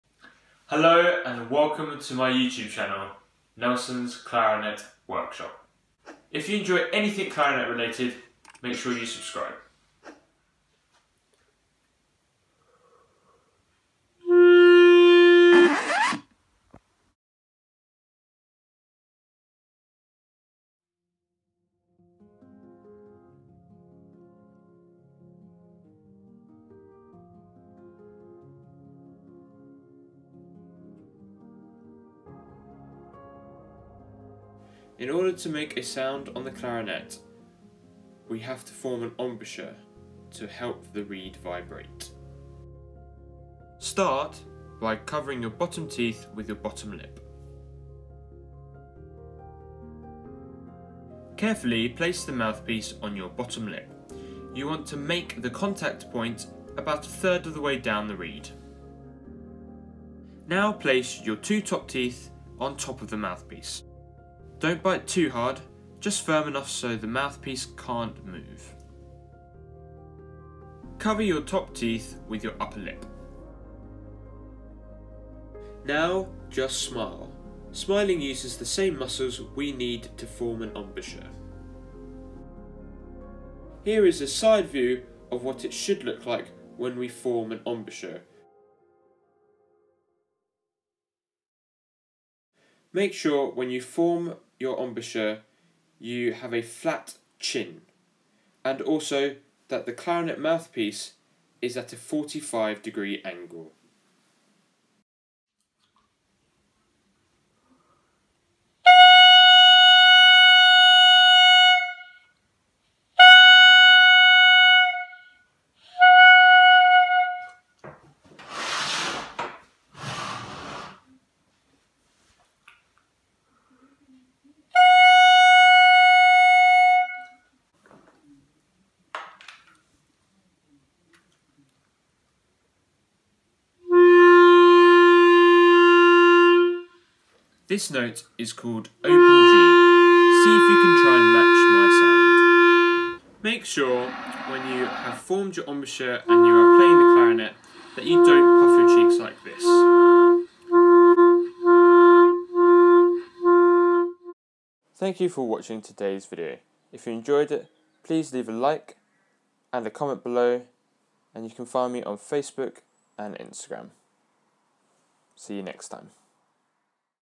How to make a sound on the clarinet in 3 minutes!
In this super short video we understand how to make a sound on the clarinet. This is achieved by forming an embouchure, a specific shape of the lips and teeth to help the reed vibrate when we blow down the instrument.